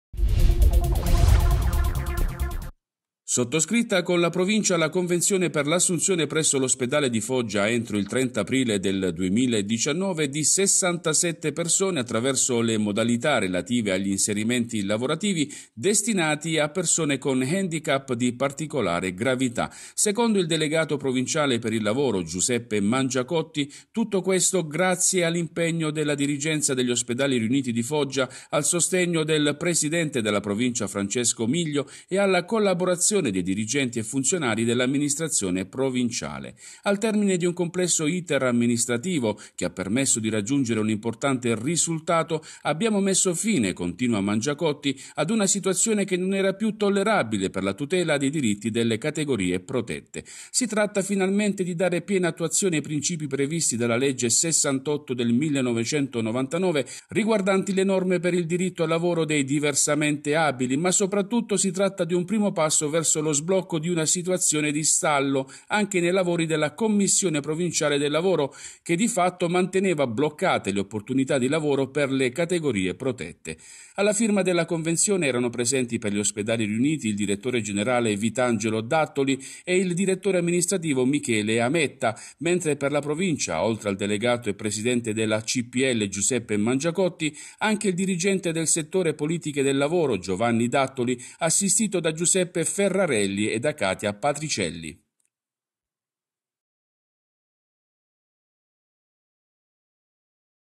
FOGGIA | FIRMATA CONVENZIONE PER L'ASSUNZIONE DI 67 DISABILI PRESSO GLI OSPEDALI RIUNITI
Sottoscritta con la Provincia la convenzione per l'assunzione presso l'Ospedale di Foggia entro il 30 aprile 2019 di 67 persone attraverso "le modalità relative agli inserimenti lavorativi destinati a persone con handicap di particolare gravità"

Secondo il delegato provinciale per il lavoro, Giuseppe Mangiacotti, tutto questo "Grazie all’impegno della dirigenza degli Ospedali Riuniti di Foggia, al sostegno del presidente della Provincia Francesco Miglio, e alla collaborazione dei dirigenti e funzionari dell’amministrazione provinciale.

Al termine di un complesso iter amministrativo, che ha permesso di raggiungere un importante risultato,
abbiamo messo fine - continua Mangiacotti - ad una situazione che non era più tollerabile per la tutela dei diritti delle categorie protette.

Si tratta finalmente di dare piena attuazione ai principi previsti dalla legge 68/99, riguardanti le norme per il diritto al lavoro dei diversamente abili, ma soprattutto si tratta di un primo passo verso lo sblocco di una situazione di stallo, anche nei lavori della Commissione Provinciale del Lavoro, che di fatto manteneva bloccate le opportunità di lavoro per le categorie protette.“

Alla firma della convenzione erano presenti per gli Ospedali Riuniti il direttore generale Vitangelo Dattoli e il direttore amministrativo Michele Ametta, mentre per la Provincia, oltre al delegato e presidente della CPL, Giuseppe Mangiacotti, anche il dirigente del settore politiche del lavoro, Giovanni D’Attoli, assistito da Giuseppe Ferrarelli e da Katia Patricelli.